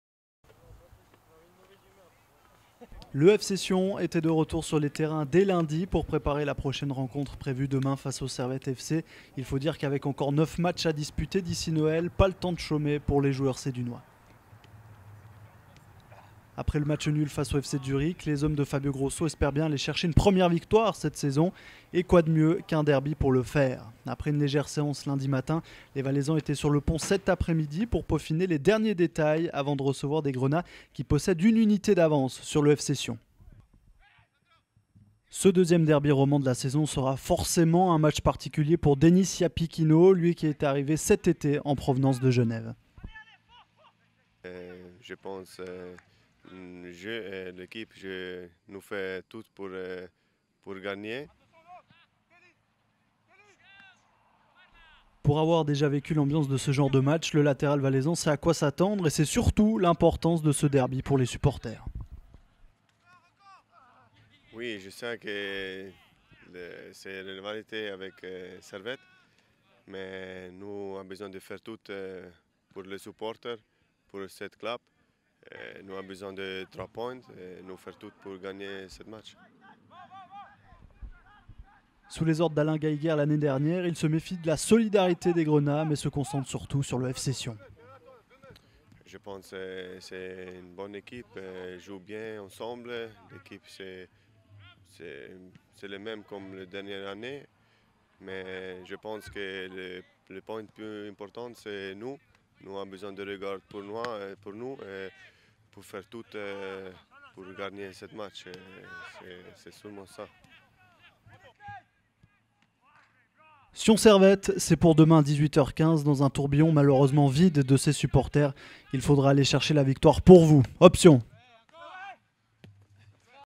Reportage avant Sion - Servette
Avant le derby du Rhône, découvrez notre reportage avec Dennis Iapichino.